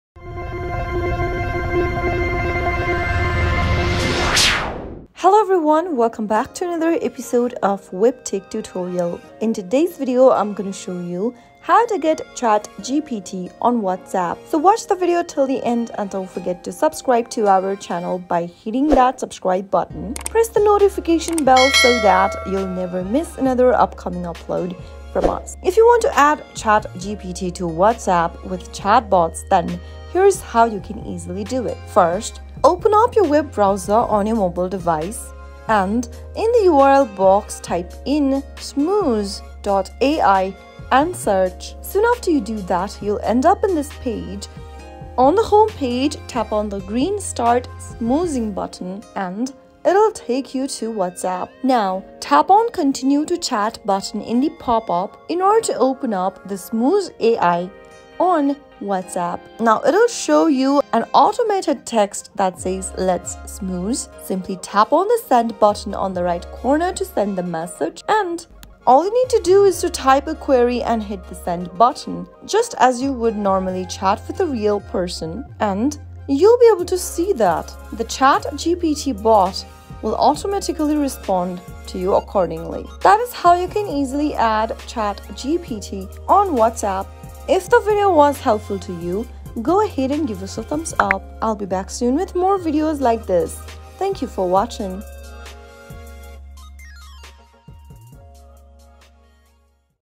How to Get Chat GPT on WhatsApp 2023?
Looking to add a touch of AI to your WhatsApp conversations? In this tutorial video, we'll show you how to get Chat GPT on WhatsApp. Chat GPT is an advanced language model that can generate realistic and contextually relevant responses, making your conversations more engaging and dynamic. Learn how to set up and integrate Chat GPT into your WhatsApp account, and discover the possibilities of AI-powered chatting. From generating witty responses to providing useful information, Chat GPT takes your WhatsApp experience to the next level. Watch now and learn how to get Chat GPT on WhatsApp!